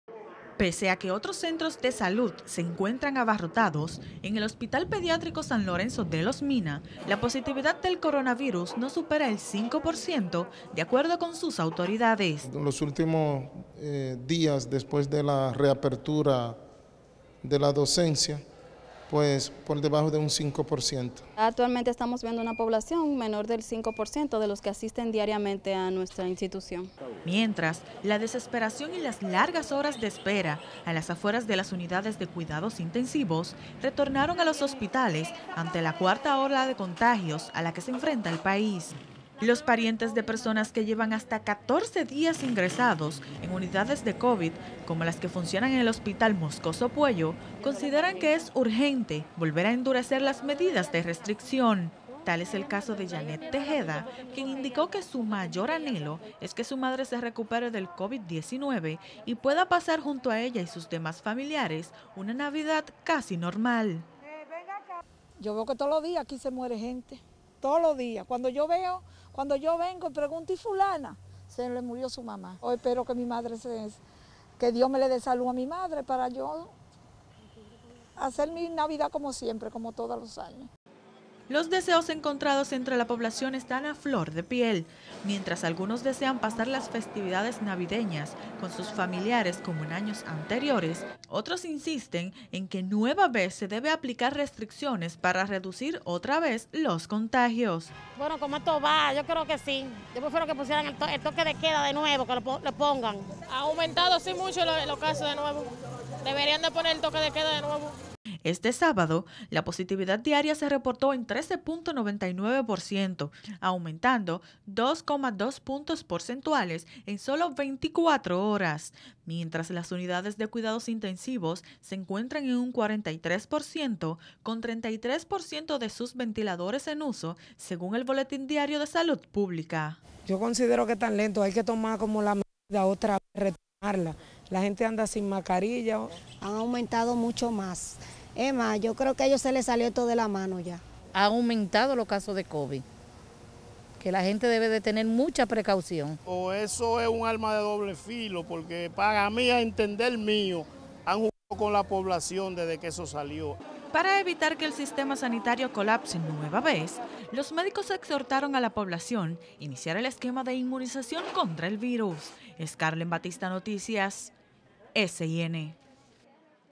Preocupa aumento de casos COVID en el país
El aumento diario de casos de covid-19, que incluye a niños ingresados en hospitales por la enfermedad y la ocupación del 43 por ciento de las unidades de cuidados intensivos, mantiene preocupada a la población que a pocos semanas de terminar el año, esperaban tener una navidad casi normal con sus familiares.